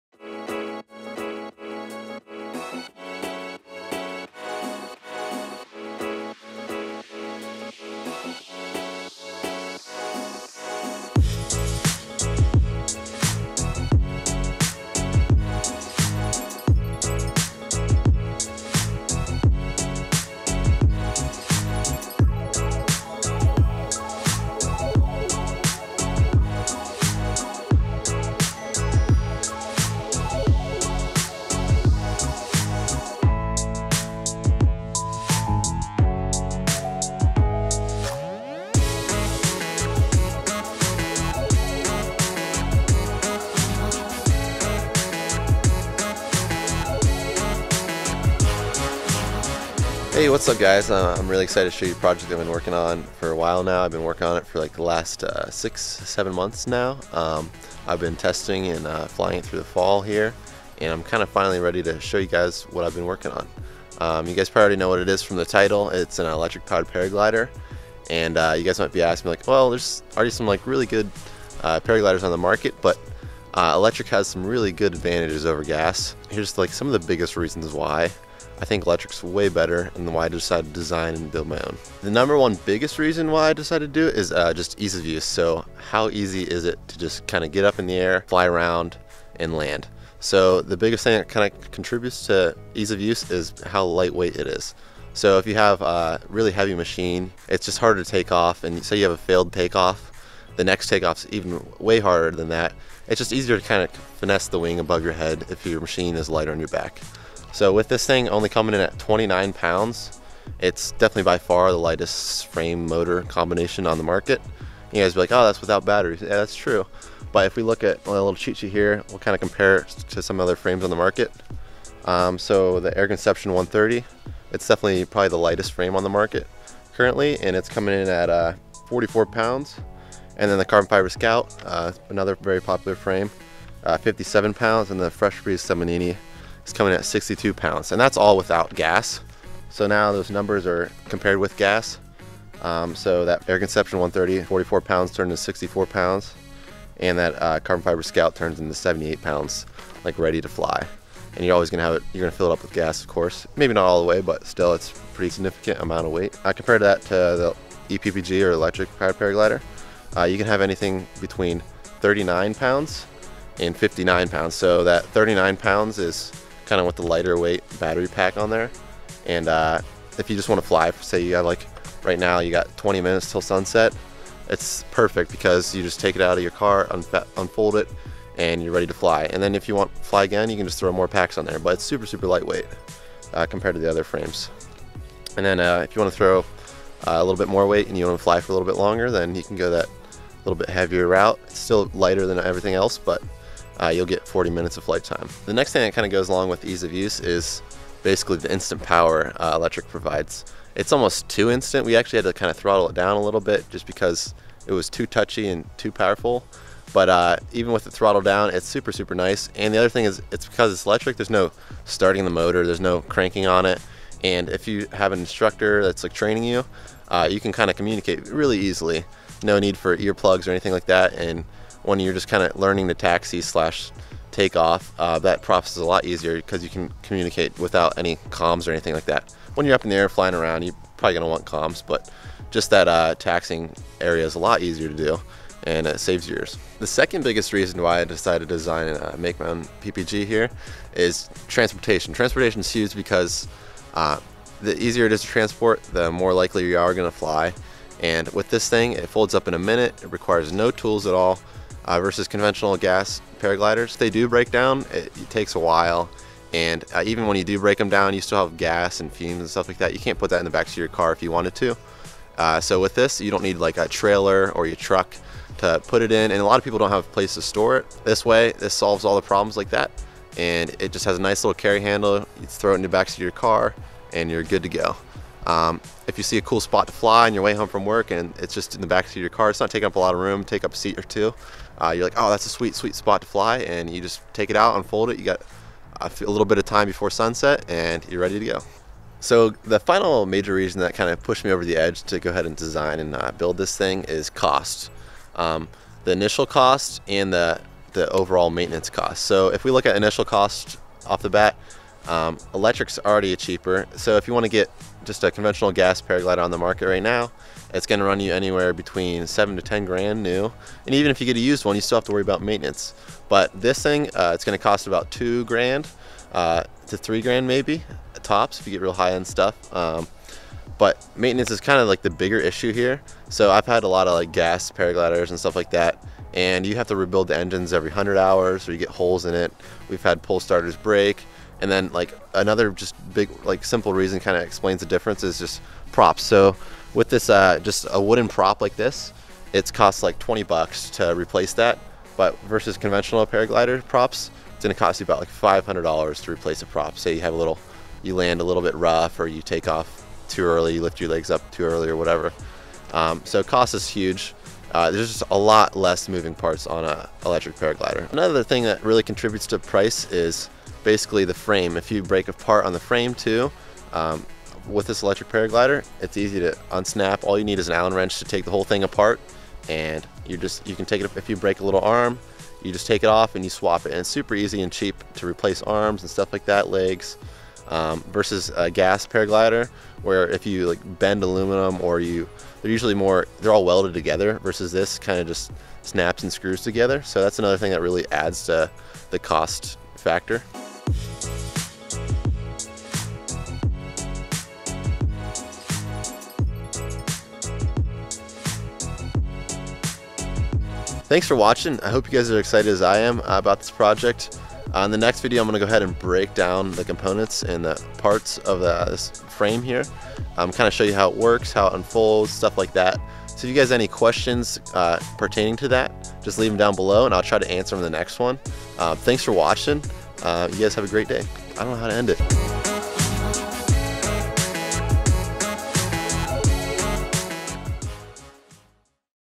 Electric Paramotor Project DIY (Vid #1)
Thanks for checking out my electric paramotor project. In the next video i'm going to break down the frame and show you the components and parts and explain how they work. If you have any questions or comments you have let me know and In the next video ill try to answer them.

If you want to you can contact use here or just put in your email for updates on the OpenPPG